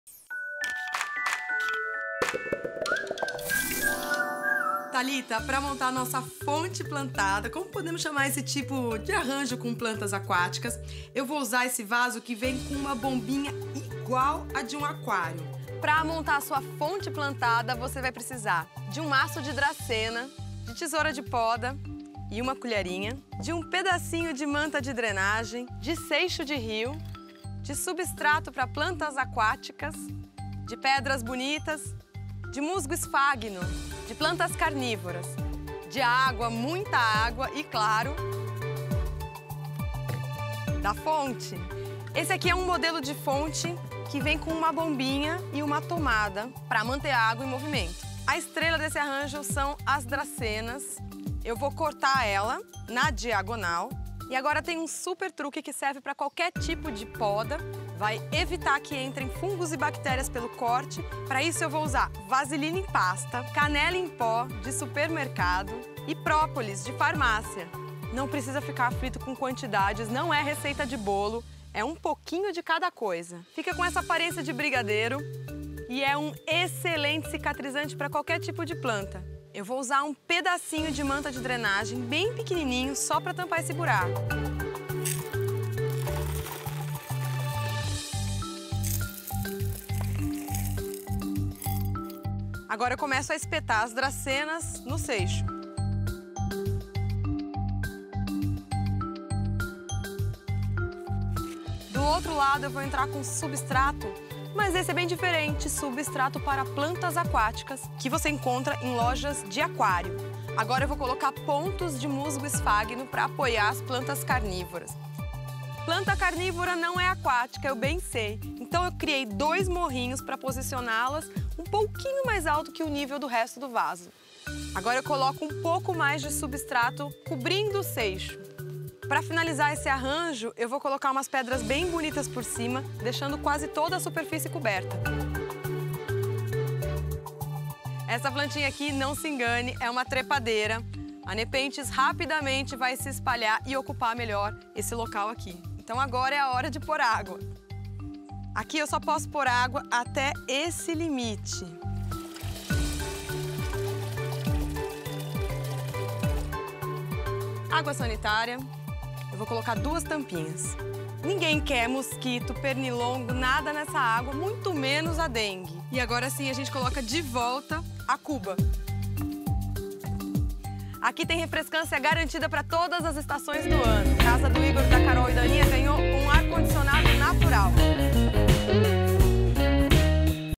Aprenda a fazer um arranjo de plantas em uma fonte de água | Jardinagem | Carol Costa
Quer inspiração para montar um cantinho zen na sua casa? Neste vídeo, a jardineira Carol Costa vai ensinar como fazer um arranjo de plantas em uma fonte de água. Além de acrescentar mais verde na decoração da sua casa, ainda tem o barulhinho da água corrente para os momentos de descanso e lazer. Confira o "Faça Você Mesmo"!

- 1 maço de dracena
- Tesoura de poda
- 1 colher pequena
- Manta de drenagem
- Seixo de rio
- Substrato para plantas aquáticas
- Pedras bonitas
- Musgo esfágno
- Plantas carnívoras
- Água
- 1 fonte

E aí, curtiu o passo a passo? Conta pra gente nos comentários, em qual cantinho da sua casa você colocaria esse arranjo de plantas?